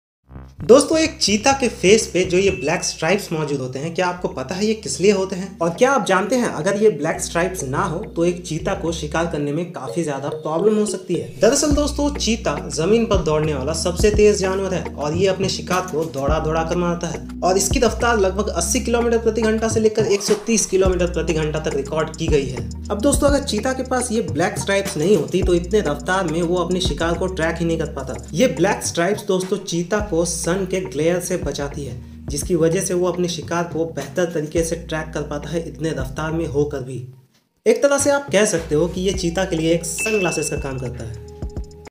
Cheetah के Face में ये काली पट्टियां क्यों है 🤔😱😱😱😎
What are the marks under a cheetahs eye ?

The dark tear mark below a cheetah's eye, called a malar stripe, Cheetahs have a way to stop the glaring sun from obstructing their view. Those black tear marks, also called malar stripes that run down from their eyes down the sides of their face, attract the sun away from the eyes. The same strategy is used by football players who put black smudges under their eyes.

So that we continue to make more videos like this for you...